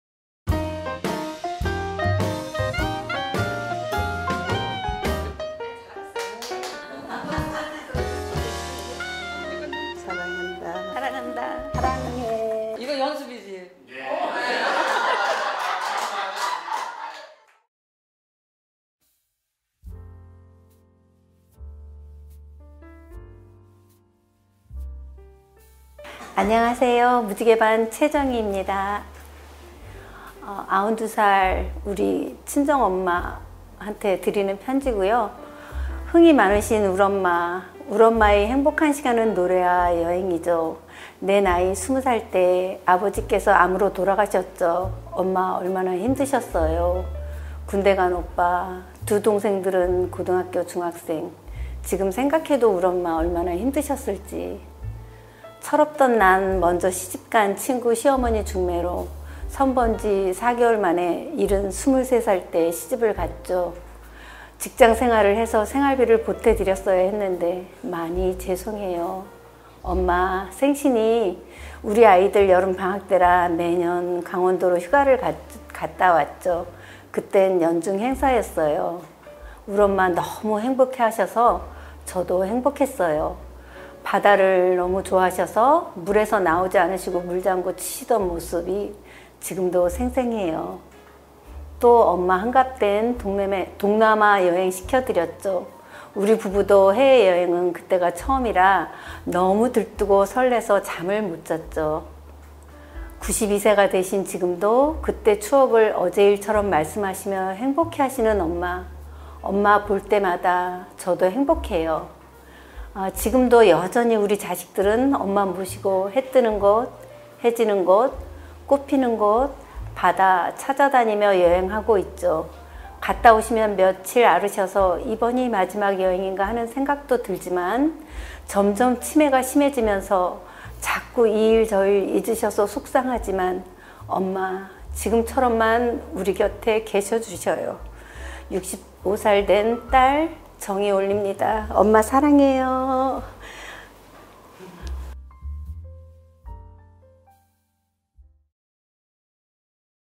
💌한 편의 편지로 남기는 나의 삶✍ 물놀이를 좋아하시던 나의 어머니께
💌한 편의 편지로 남기는 나의 삶✍
최정희님이 어머니께 보내는 삶 이야기입니다 :)

65세 이상의 어르신들로 구성 된 [다시 가는 학교 :  7학년 교실]
자기효능감과 자기돌봄을 위한 🙌
1년의 여정을 시작하고
벌써 반년이 지나갔어요🥳

그 첫번째 결과물인
"한 편의 편지로 남기는 나의 삶" 시리즈를 공개합니다✨

나의 삶을 돌아보고
다른 세대에 전하는 응원이 담긴
특별한 영상편지💌

서울시평생교육진흥원은 전 세대의 평생학습을 응원합니다 :)